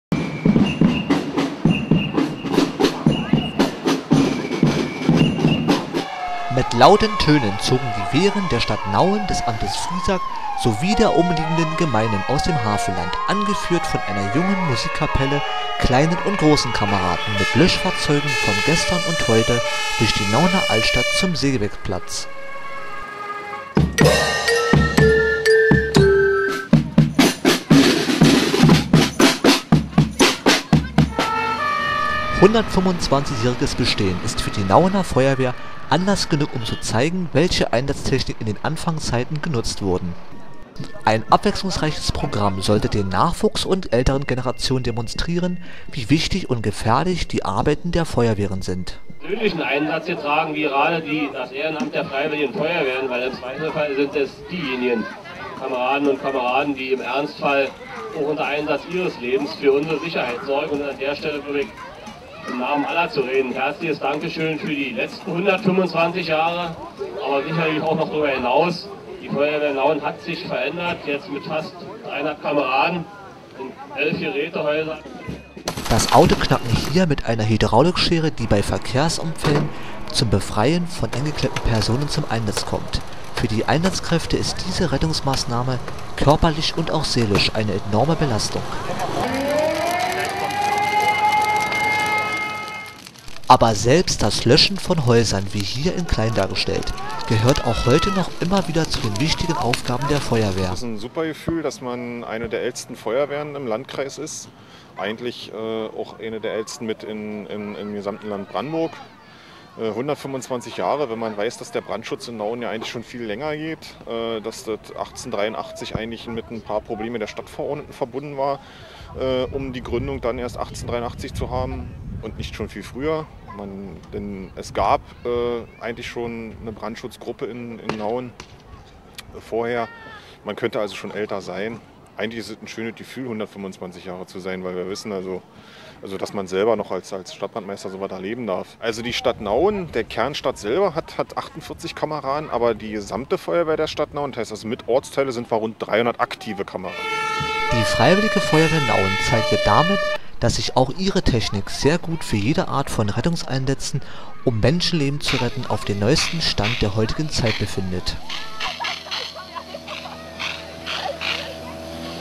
Feuerwehr Nauen 125. jähriges bestehen
Einsatzfahrzeuge aus früheren Zeiten wie der Wartburg 3.53

Über 35. Löschfahrzeuge zogen gemeinsam mit etwa 150. Kameraden der
Wehren durch die Altstadt. Für gutes Programm war ebenfals gesorgt
mit Stolz präsentierten die freiwilligen Ihr können mit der Technik.